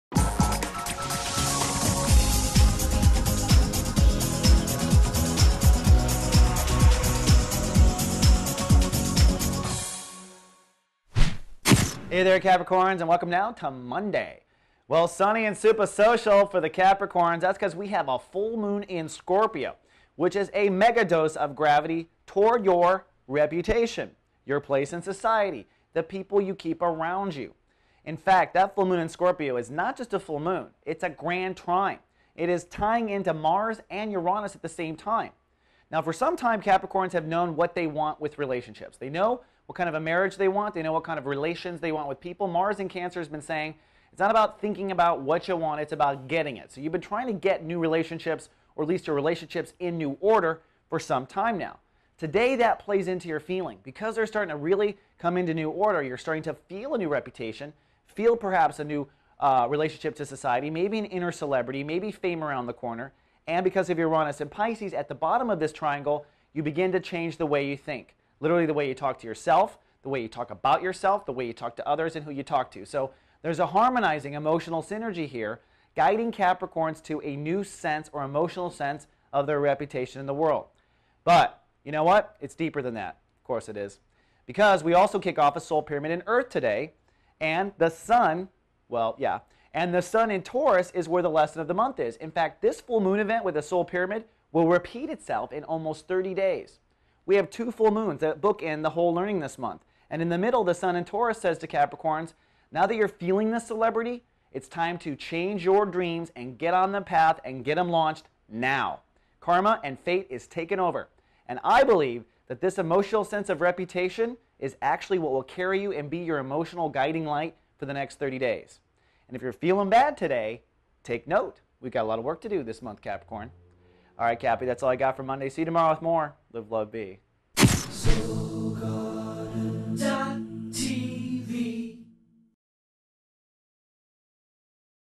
Capricorn - 4/21/2008 Soul Horoscopes
Your Daily Video Horoscope forecast presented by Stand Up Astrologer Christopher Witecki and Soulgarden.tv